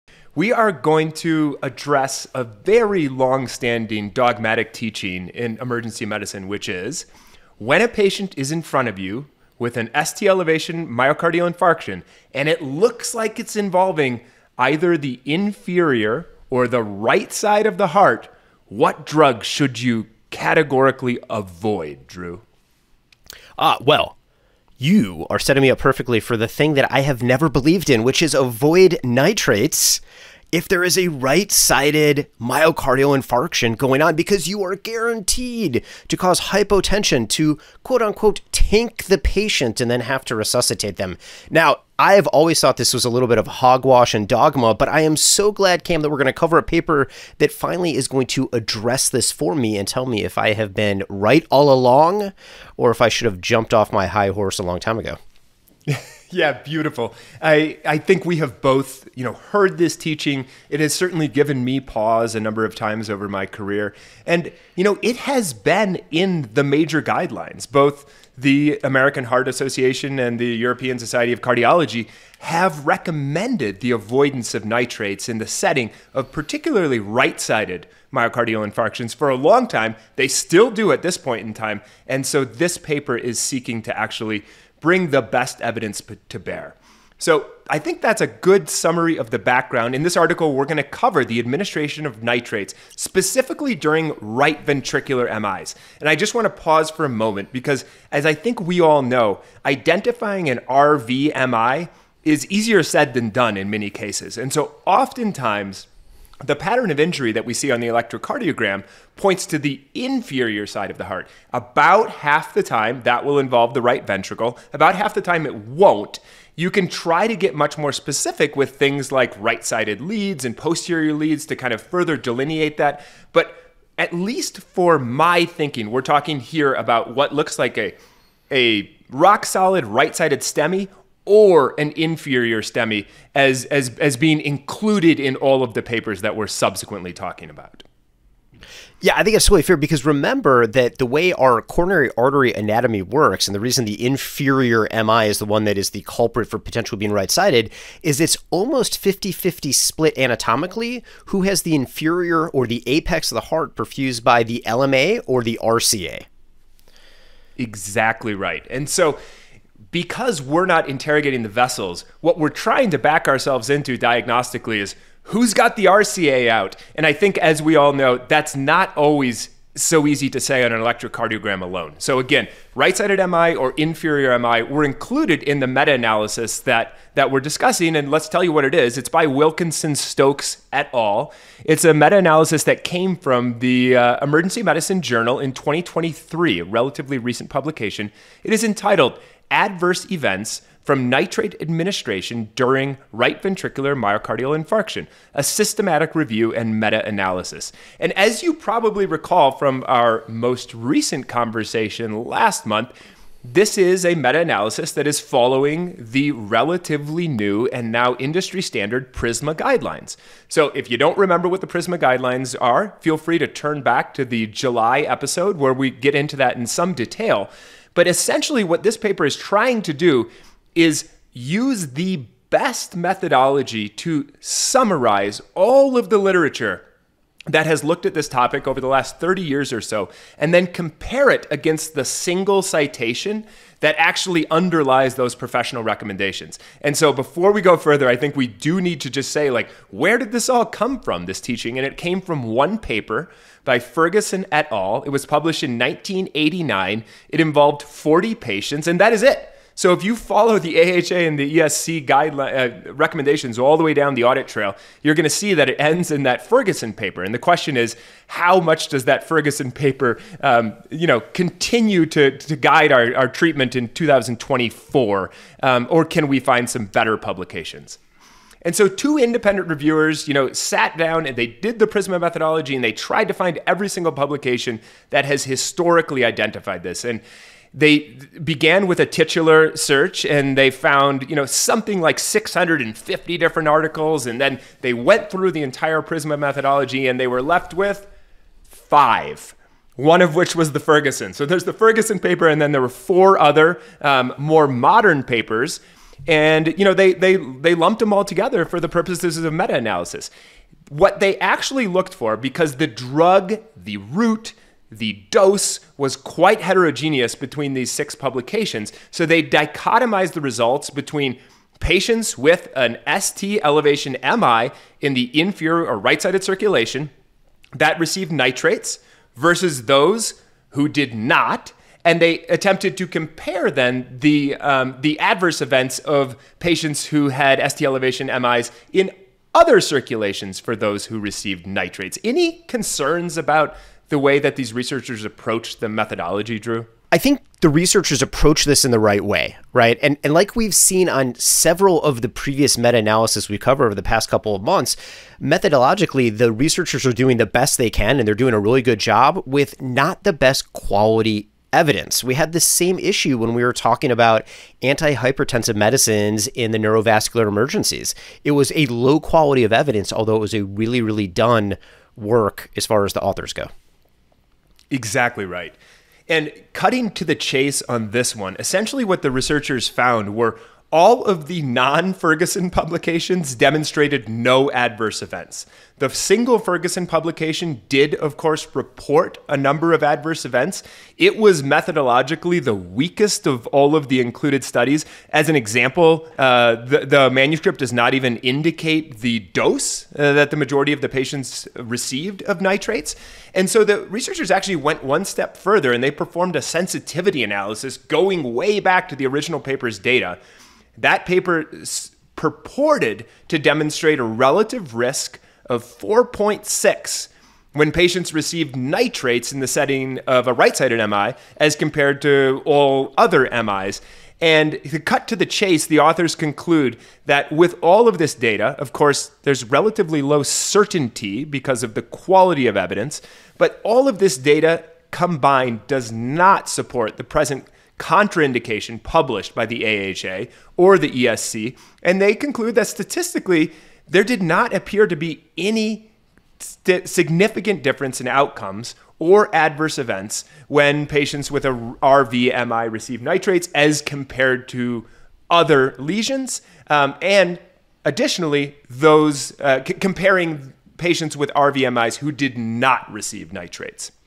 ERcast - Lit Matters: What If We Give Nitrates To A Right-Sided MI?
Is it time to let sleeping dogma lie? Historically we've been taught to avoid using nitrates if a patient has signs of a right-sided MI. Watch as Drew Kalnow and Cam Berg pull back the curtain on this long-held teaching and find a simple, evidence-based path forward!